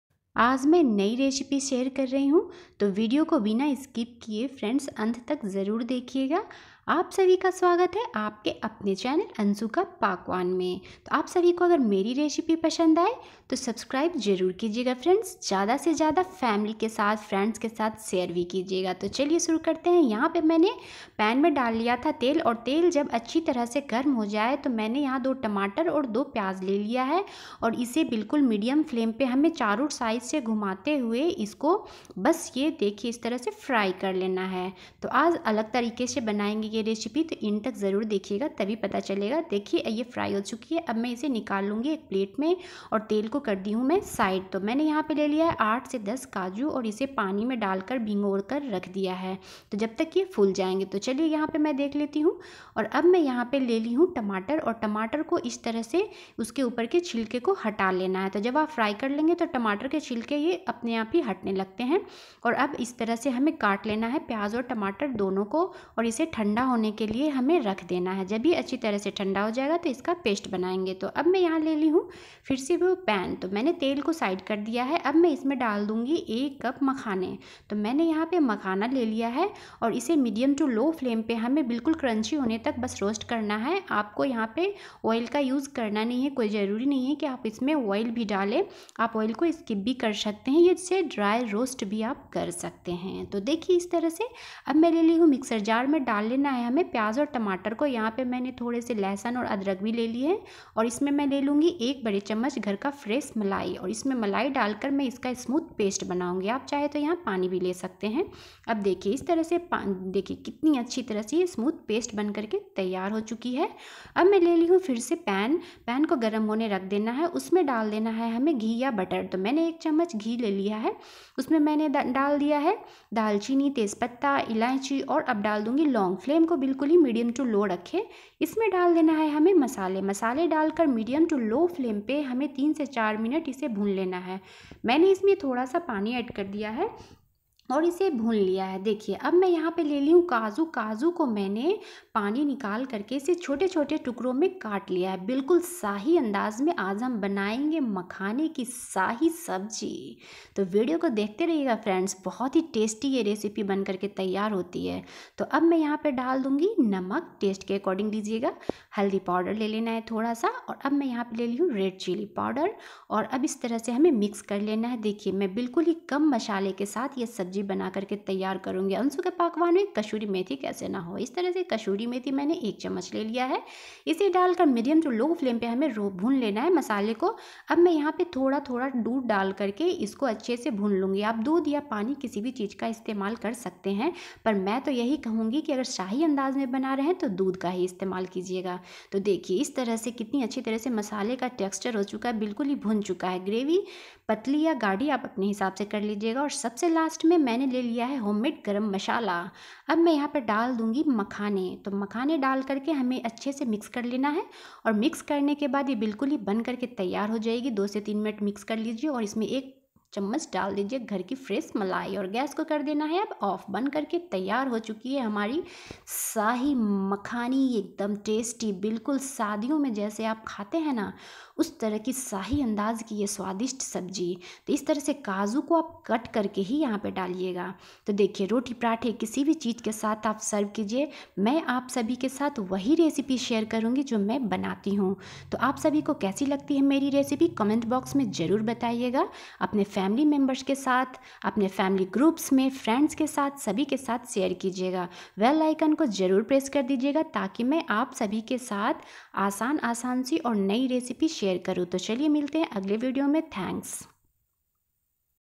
इस रेसिपी का कोई जोड़ नहीं बिल्कुल शाही अंदाज में बनाएं मखाने की स्पेशल रेसिपी | anshu ka pakwan
इस रेसिपी का कोई जोड़ नहीं बिल्कुल शाही अंदाज में बनाएं मखाने की स्पेशल रेसिपीMakhana anshu ka pakwan

Hello friends,
aaj main aap Sabhi Ke Sath makhane ki tasty recipe share ki hun.
Bilkul sahi tarike se banaen makhane ki bhi recipe.
🔔icon ko Jarur press kar dijiyega Taki meri har video ki notification aap Sabhi Ke Pass Jayegi.